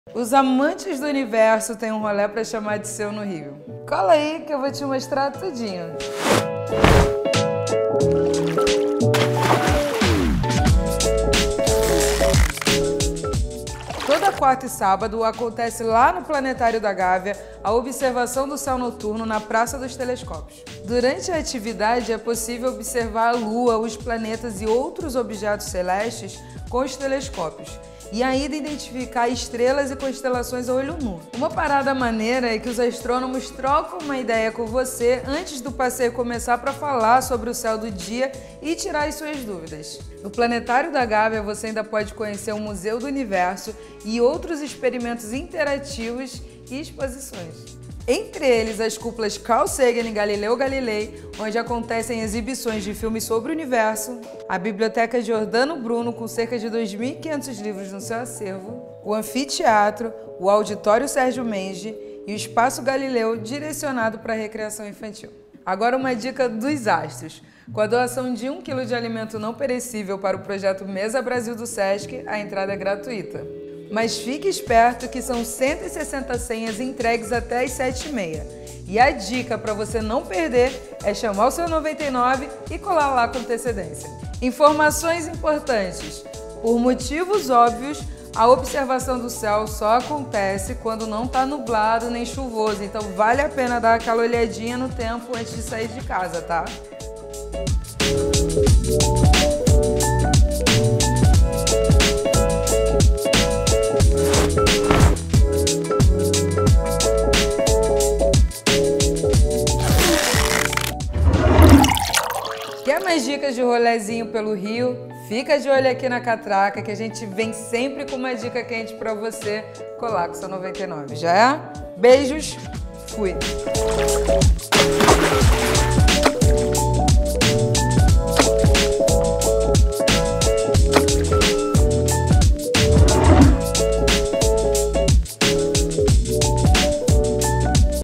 Vem ver o céu à noite no Planetário da Gávea no Rio
Os amantes do universo têm um rolé para chamar de seu no Rio!

Toda quarta e sábado acontece lá no Planetário da Gávea a observação do céu noturno na reformada Praça dos Telescópios.

Colai que eu vou te contar tudinho!